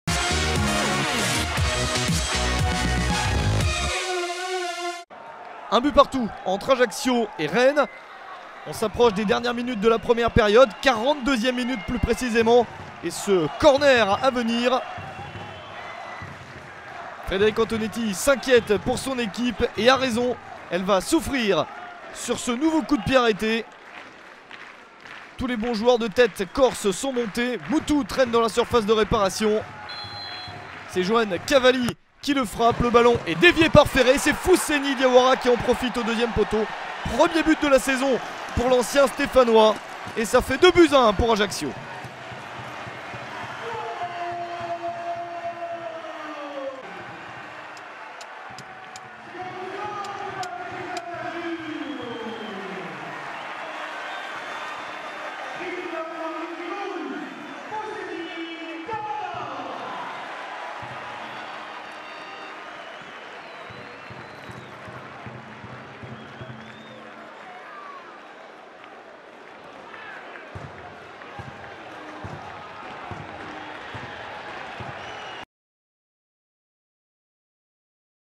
But Fousseni DIAWARA (42') - AC Ajaccio - Stade Rennais FC (2-4) / 2012-13
AC Ajaccio - Stade Rennais FC (2-4) - But de Fousseni DIAWARA (42'). Tous les buts de AC Ajaccio - Stade Rennais FC en vidéo.

Stade François Coty - samedi 22 décembre 2012

Buteurs : Adrian MUTU (2' - AC Ajaccio) / Julien FERET (17' pen - Stade Rennais FC) / Fousseni DIAWARA (42' - AC Ajaccio) / Romain ALESSANDRINI (44' - Stade Rennais FC) / Cheick DIARRA (60' - Stade Rennais FC) / Julien FERET (72' - Stade Rennais FC)